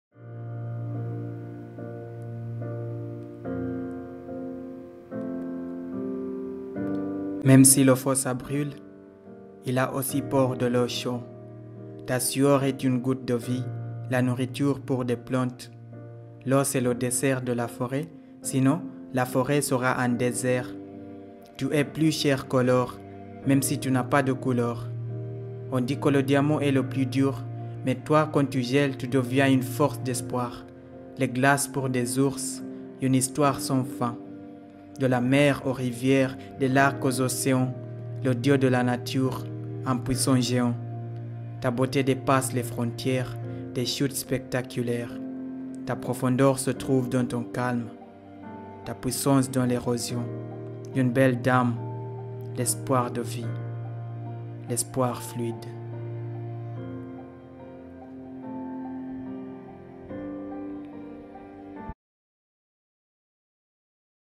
L'ESPOIR FLUIDE
A spokenword about the importance and power of water.
Un slam poesie sur la puissance d'eau.